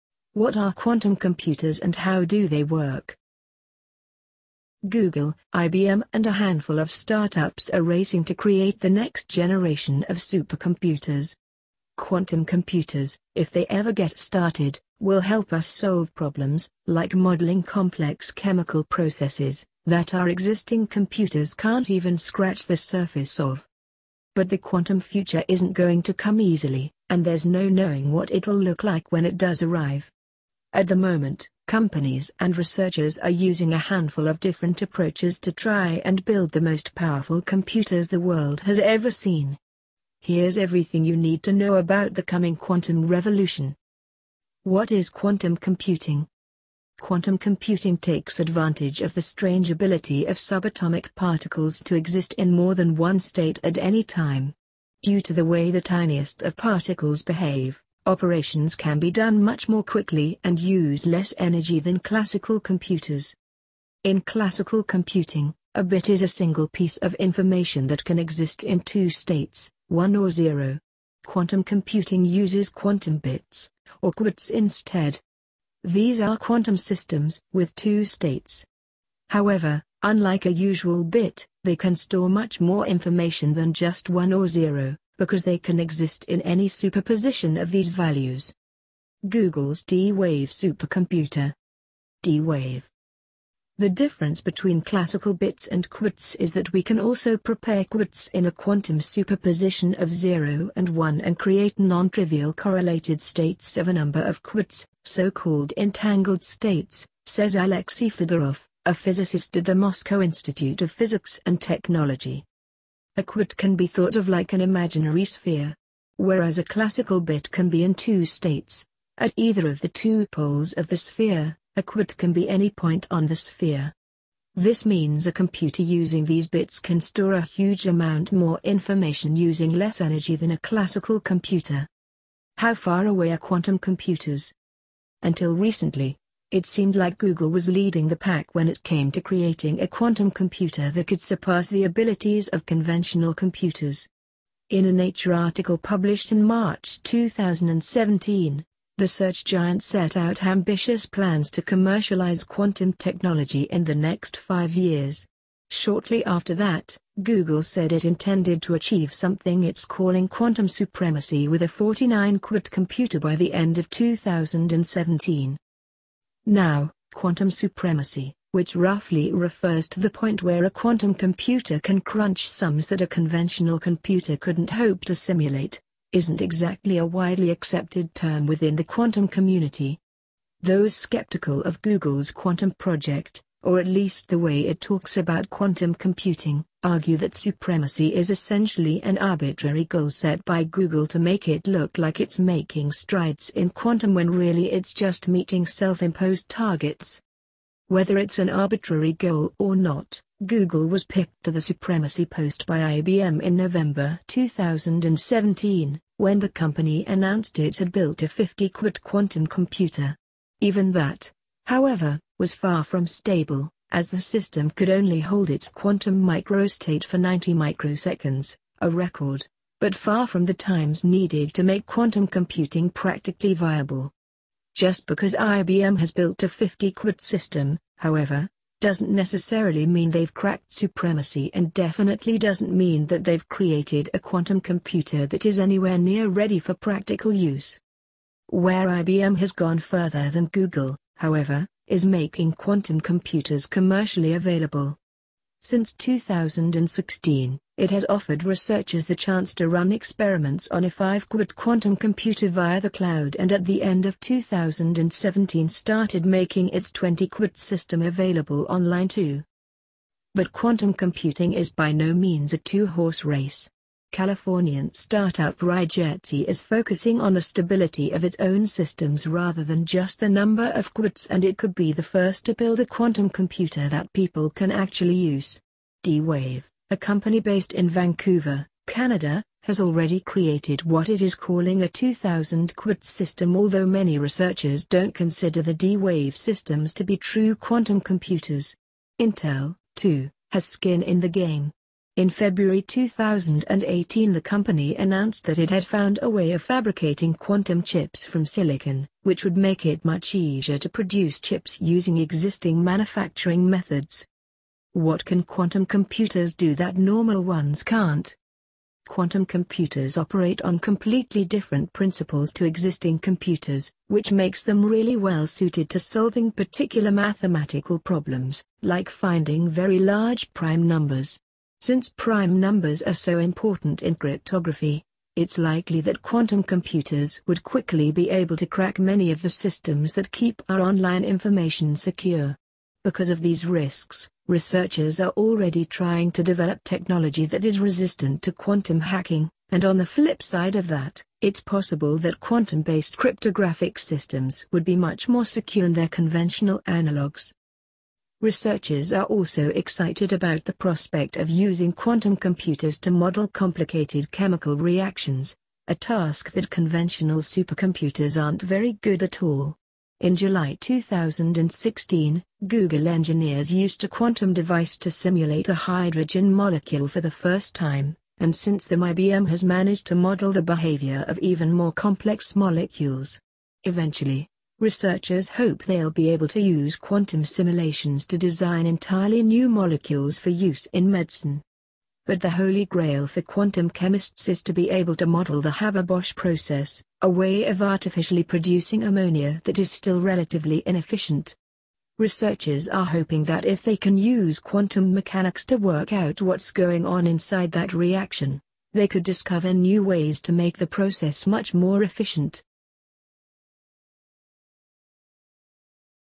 How does QUANTUM computer WORK?
How do quantum computers operate and what is qubit? What makes them incredibly fast and unsafe? Greater processing speed also imposes greater security risk..